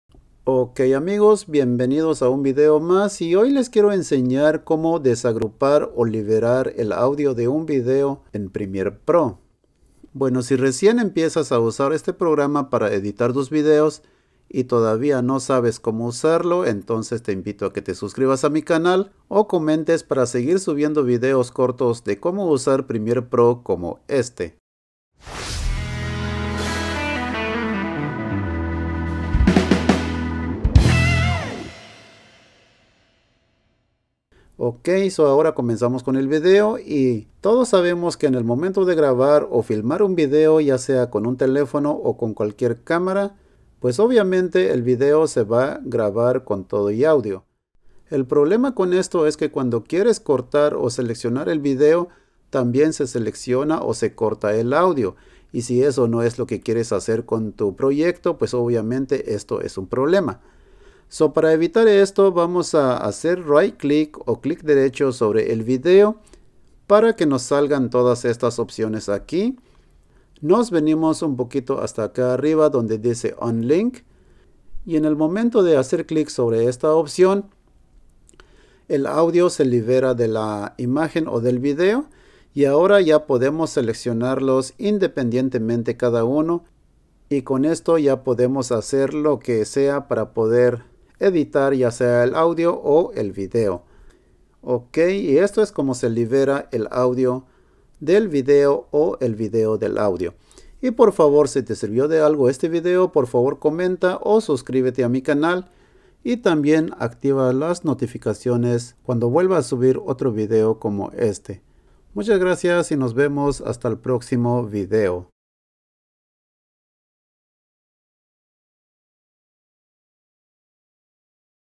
Como separar el audio de un video en Primiere Pro 2021
Como separar o liberar el audio de un video clip en Primiere Pro.
Cuando un video tiene su audio original de la filmación, no se separan automático en Premiere Pro porque el audio y el video son uno solo originalmente. En este video te voy a enseñar como separarlos para que se pueda editar cada uno independientemente en Premiere Pro.